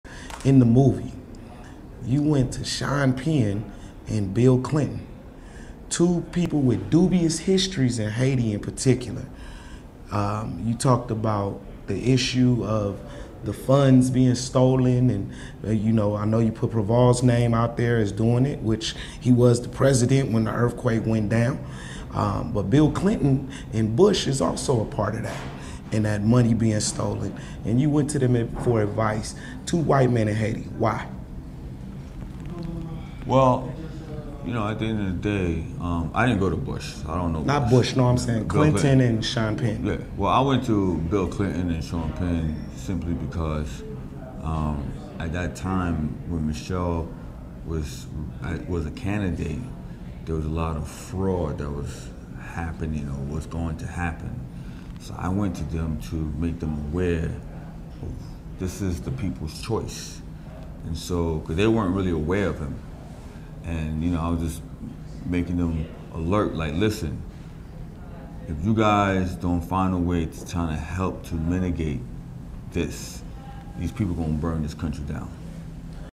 Unraveling Haiti's Political Fraud: Bill Clinton and Sean Penn advising Pras of the Fugees.
🎥 Uncover the complexities of Haiti's post-earthquake recovery and political dynamics in a candid interview with Pras from the legendary Fugees on Block Report TV! 🔥

🌐 Join us for this eye-opening conversation as we navigate through the challenges of Haiti's political landscape and the quest for recovery.

📣 Share your thoughts on the complexities of Haiti's recovery in the comments below and let's continue the dialogue!